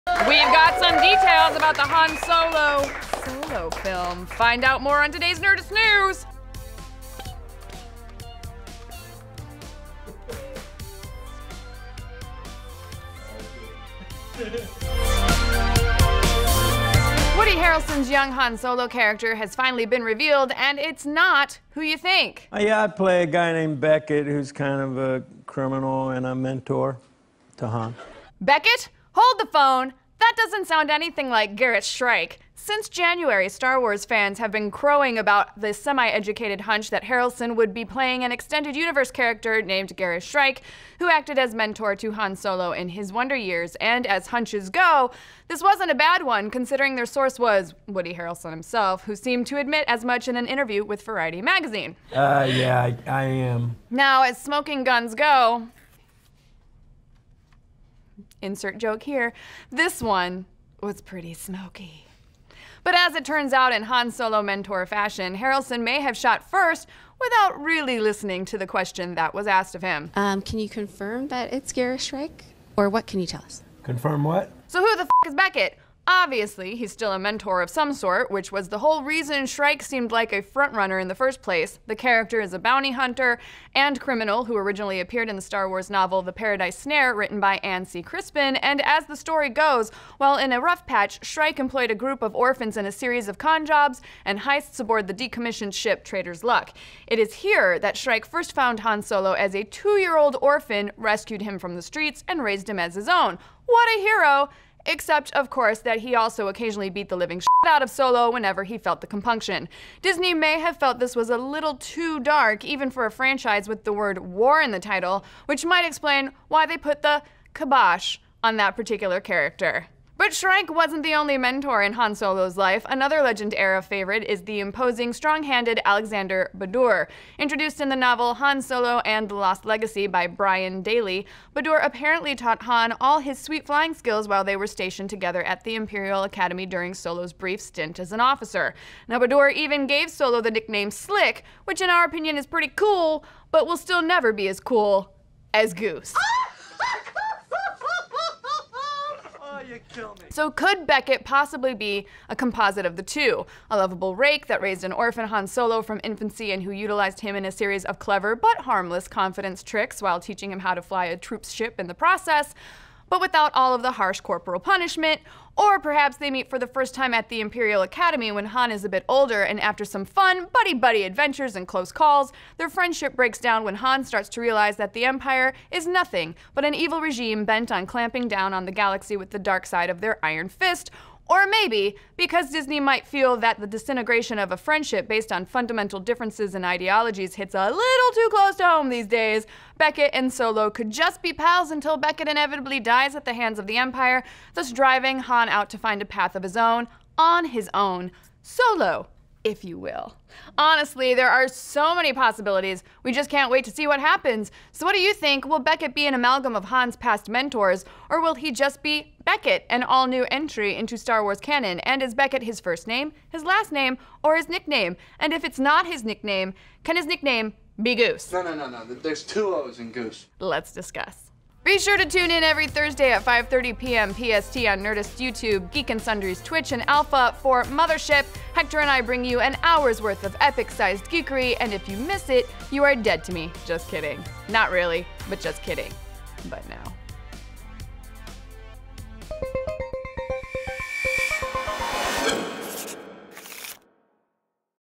Woody Harrelson’s Real Young Han Solo Character Revealed! (Nerdist News w/ Jessica Chobot)
If you thought you knew who Woody Harrelson was playing in the Star Wars universe, you’re wrong because thanks to his appearance on the Tonight Show, we now know the truth! Jessica has the reveal and more on today's Nerdist News!

Are you excited to see him play this character?

Image: Lucasfilm